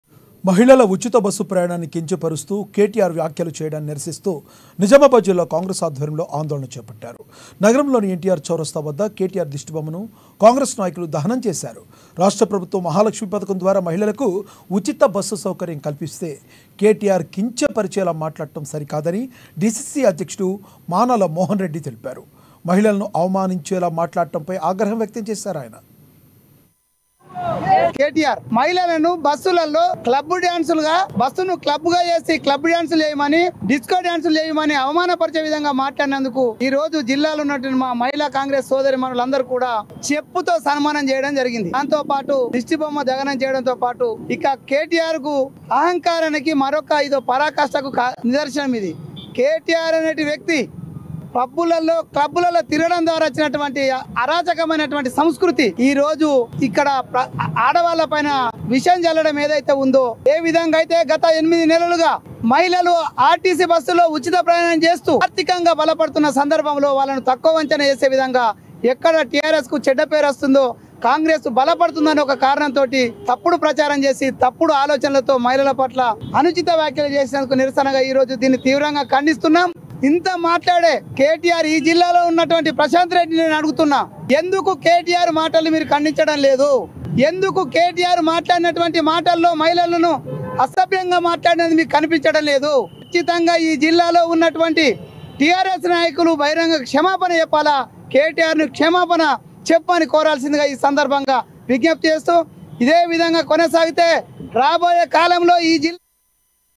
Nizamabad : KTR చేసిన వ్యాఖ్యలపై నిజామాబాద్ లో కాంగ్రెస్ నేతల ధర్నా | Congress Leaders Dharna
Nizamabad : KTR చేసిన వ్యాఖ్యలపై నిజామాబాద్ లో కాంగ్రెస్ నేతల ధర్నా | Congress Leaders Dharna | Amma News